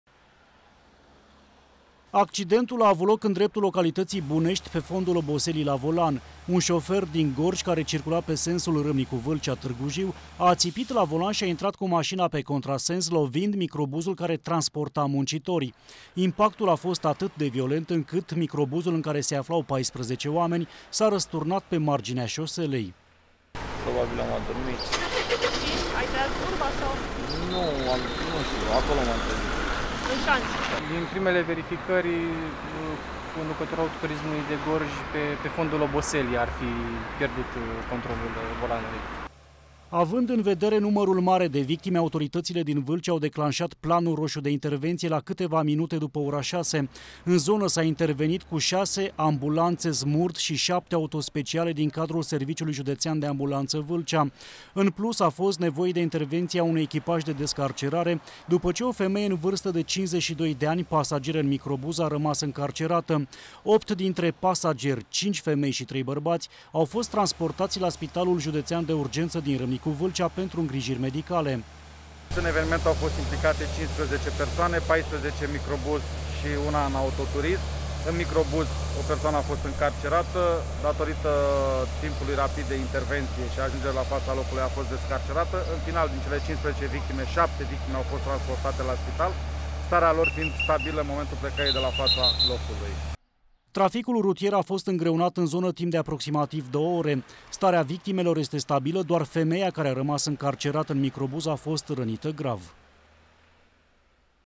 MICROBUZ CU 14 OAMENI, RĂSTURNAT ÎN VÂLCEA
Grav accident de circulaţie pe Drumul Naţional 67, în judeţul Vâlcea. Autorităţile au declanşat Planul Roşu de intervenţie după ce un microbuz cu 14 oameni s-a răsturnat în şanţ. Şoferul microbuzului a încercat să evite o coliziune frontală cu un autoturism care a pătruns pe contrasens. Din primele cercetări de vină pentru producerea accidentului este un tânăr din judeţul Gorj care a aţipit la volan.